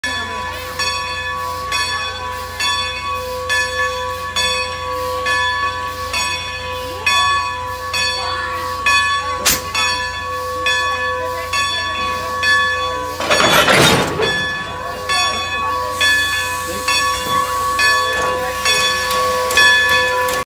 Coupling Trains Together! Steam Locomotive In Strasburg Pennsylvania
Steam locomotive coupling to a train in Strasburg, Pennsylvania on the Strasburg Railroad.